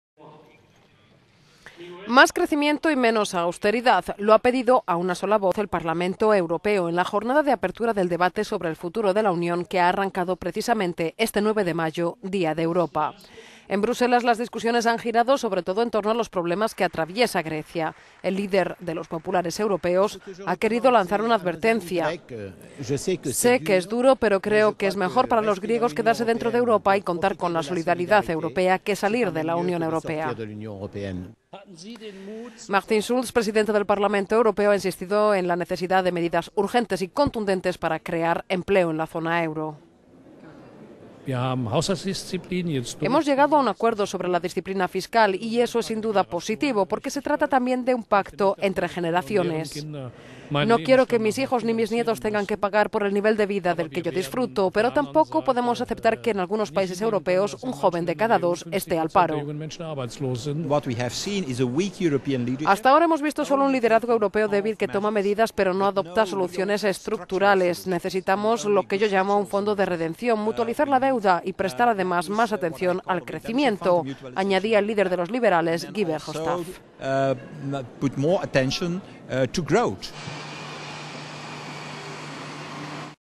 Debate sobre el futuro de la Unión
Más crecimiento y menos austeridad. Lo ha pedido, a una sola voz, el Parlamento europeo en la jornada de apertura del debate sobre el futuro de la Unión que ha arrancado precisamente este nueve de mayo, Día de Europa.
En Bruselas las discusiones han girado sobre todo en torno a los problemas que atraviesa Grecia.

El líder de los populares europeos ha querido lanzar una advertencia: "Sé que que es duro", ha dicho Joseph Daul, "pero creo que sea mejor para los griegos quedarse dentro de Europa y poder contar con la solidaridad europea que salir de la Unión Europea"

Martin Schulz, presidente del Parlamento Europeo, ha insistido en que la necesidad de medidas urgentes y contundentes para crear empleo en la zona euro. "Hemos llegado a un acuerdo sobre la disciplina fiscal, y eso es sin duda positivo. Porque se trata también de un pacto entre generaciones. No quiero que mis hijos ni mis nietos tengan que pagar por el nivel de vida del que yo disfruto. Pero tampoco podemos aceptar que en algunos países europeos un joven de cada dos este al paro", decía Schulz.

"Hasta ahora hemos visto solo un liderazgo europeo débil, que toma medidas pero no adopta soluciones estructurales. Necesitamos lo que yo llamo un fondo de redención, mutualizar la deuda. Y prestar, además, más atención al crecimiento", añadía el líder de los liberales, Guy Verhostadt.